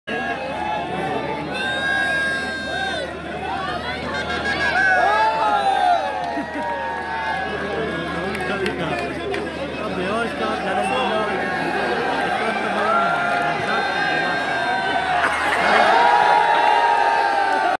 Yasir Shah Funny During Pratice Session
Yasir Shah Funny During Pratice Session With fielding coach grant Bradburn